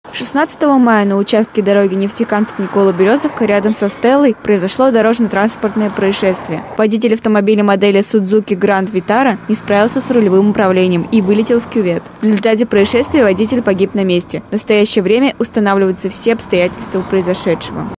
Смертельное ДТП на трассе Нефтекамск-Николо-Березовка
В субботу у стелы на въезде в Николо-Березовку перевернулся автомобиль. Водитель погиб на месте происшествия.